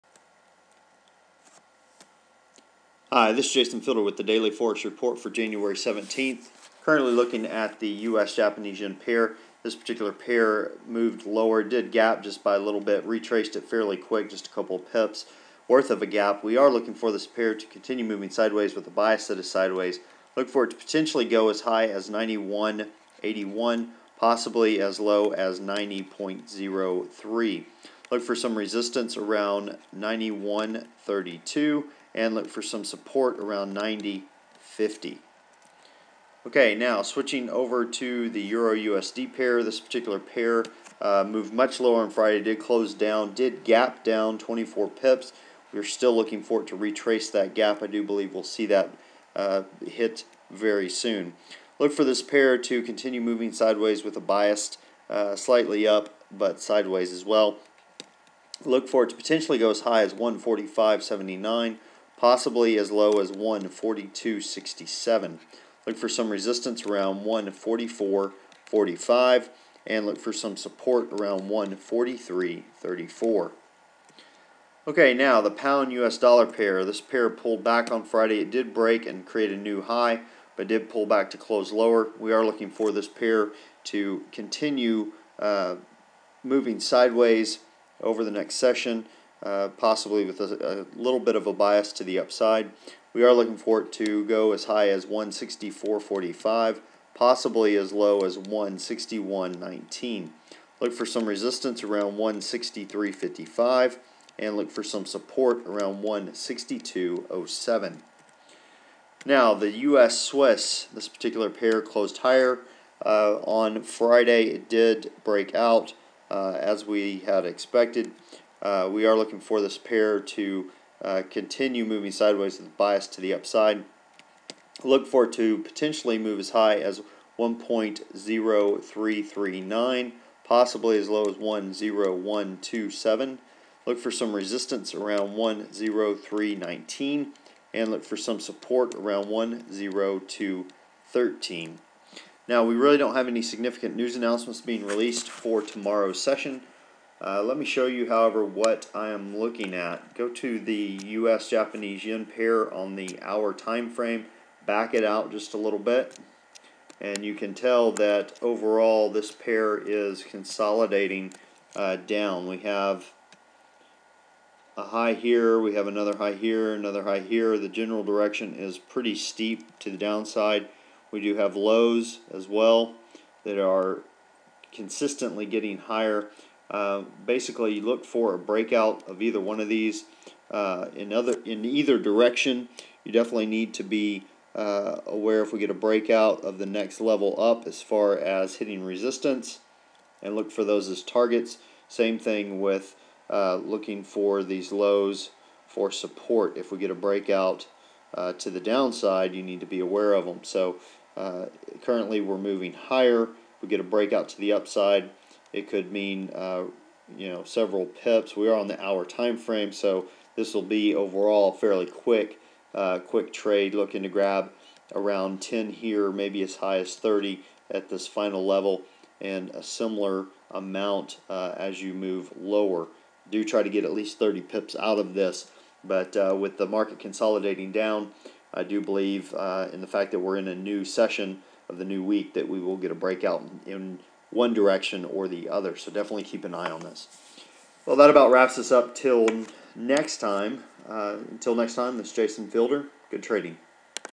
Consolidation...
There is a pair that is consolidating down on the one hour chart and is ready to breakout. The nice thing about this setup is that you can profit regardless of the direction of the breakout...